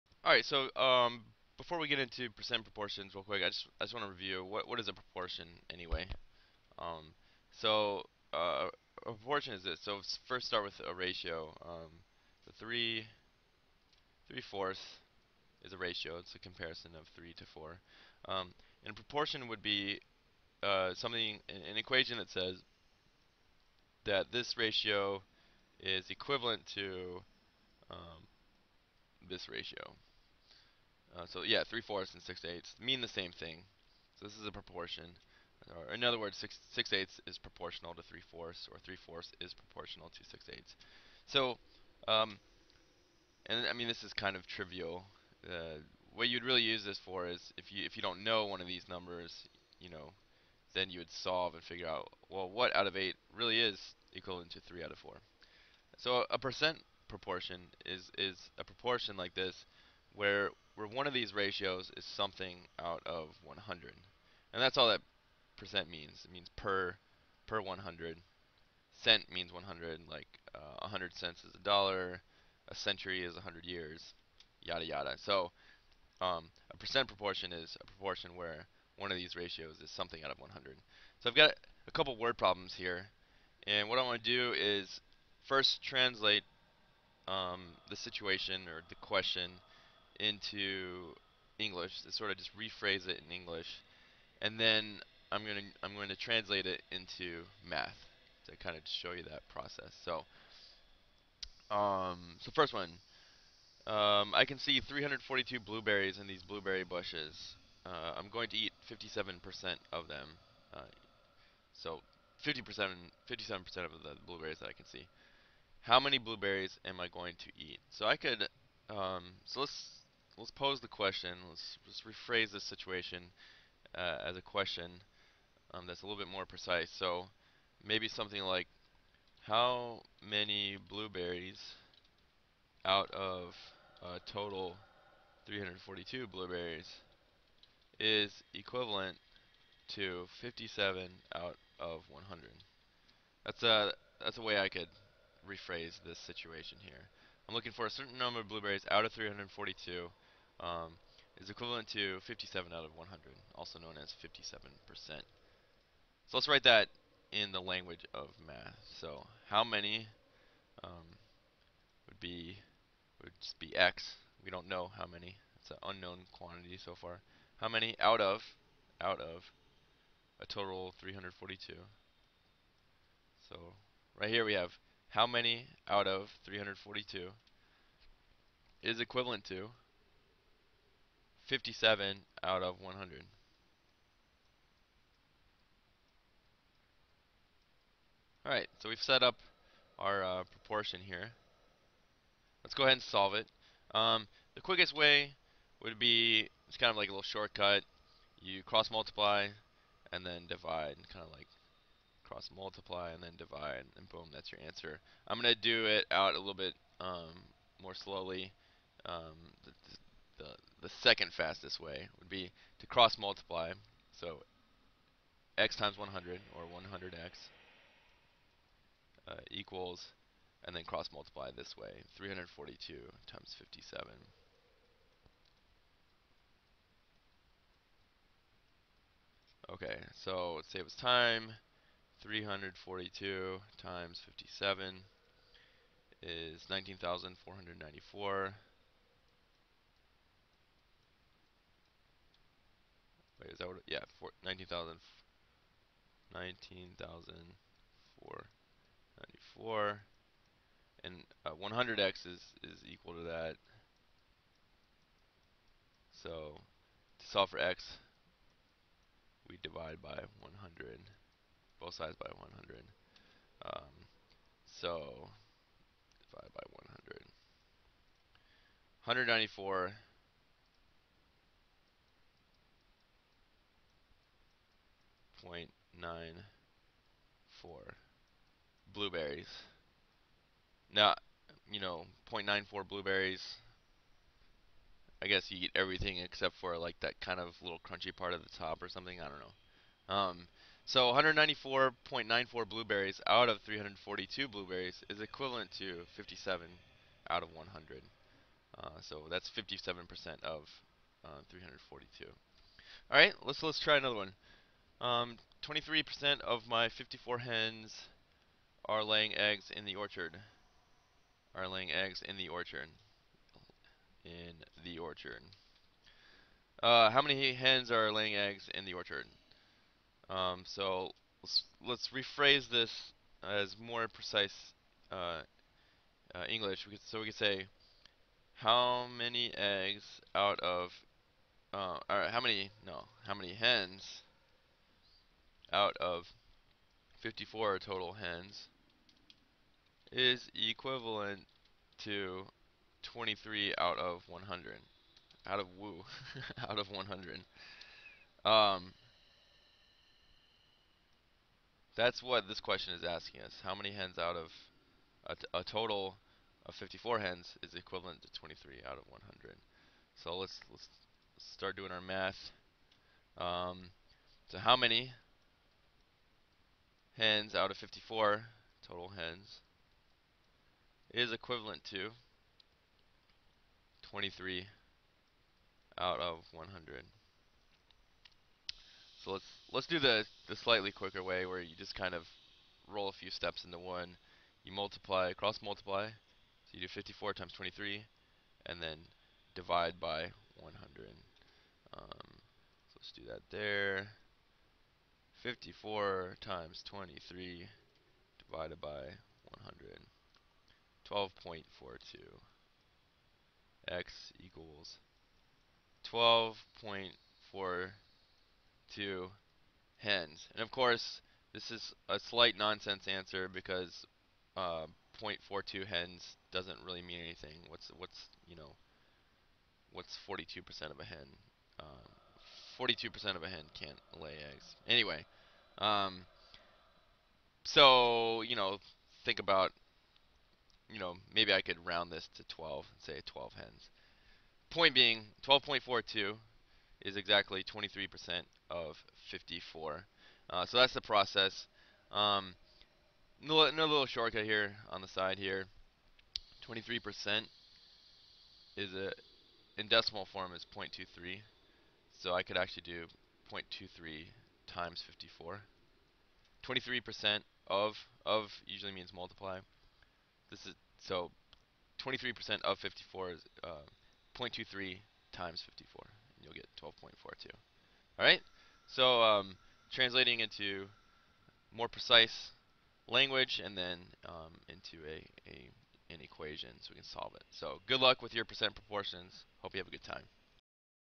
How to Translate Percent Proportion Problems Into Clearer English and Even Clearer Algebra
In this lesson, I'll show you how to take a couple different kinds of percent proportion word problems and translate them, first into more precise English phrases, and then into the language of math, so you can solve them!